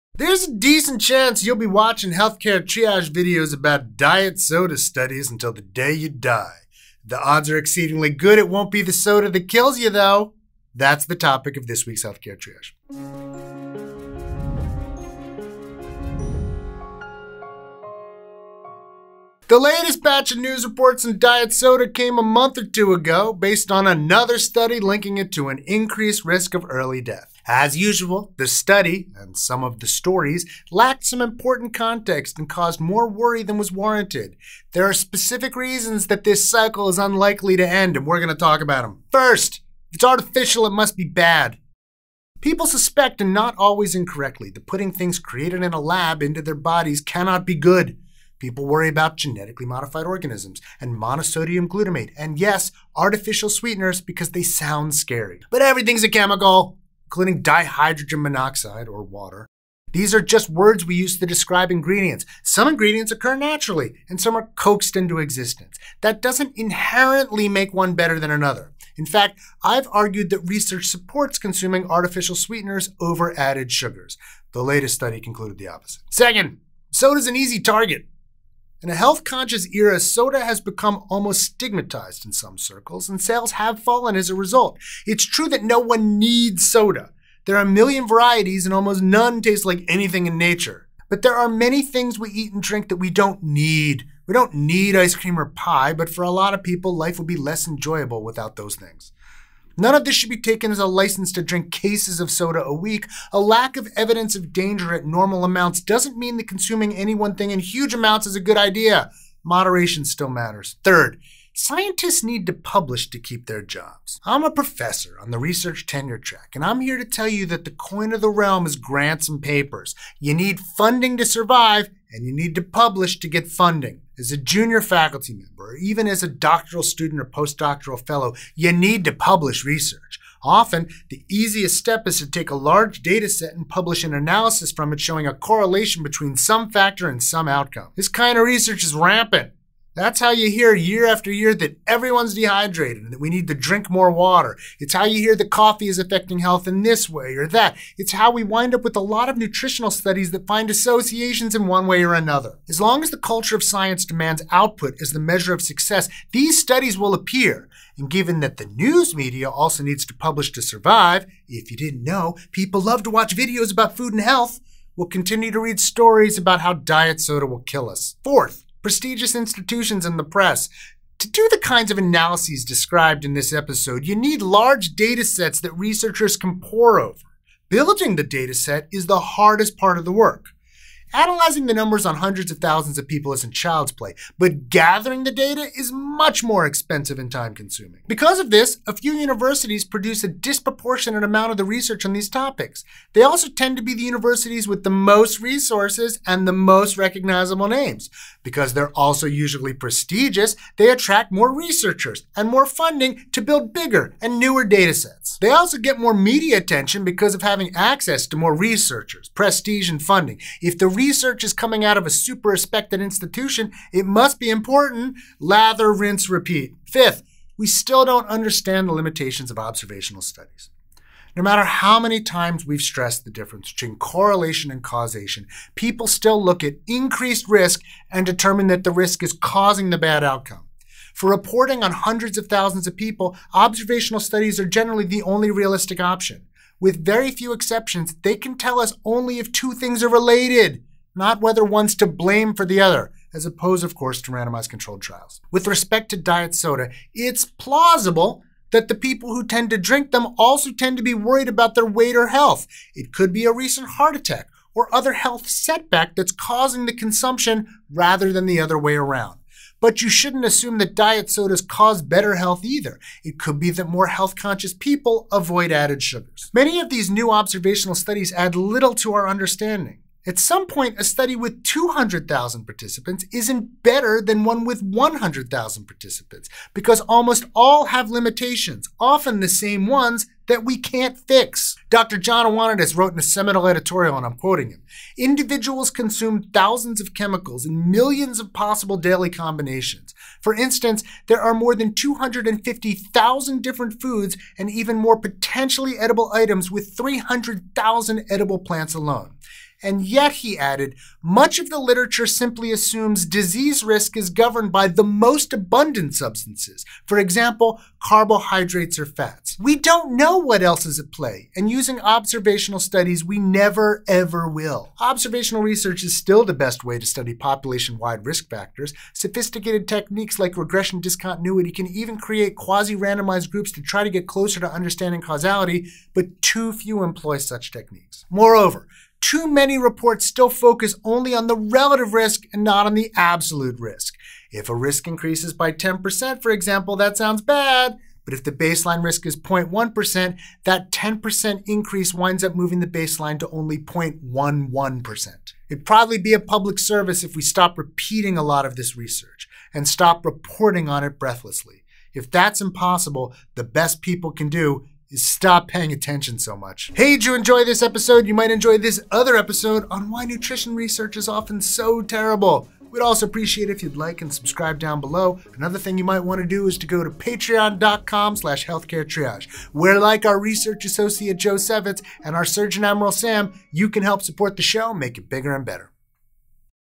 The Diet Soda Myth and Barriers to Good Research
A recent study had a lot of negative things to say about diet sodas, but how seriously should we take that study? Observational research can be powerful and useful, but it can also lead to shaky outcomes. So, what does this study tell us about some of the limitations in how research is done?

Other Healthcare Triage Links:
2.
3.
4. Aaron's book "The Bad Food Bible: How and Why to Eat Sinfully" is available wherever books are sold, such as Amazon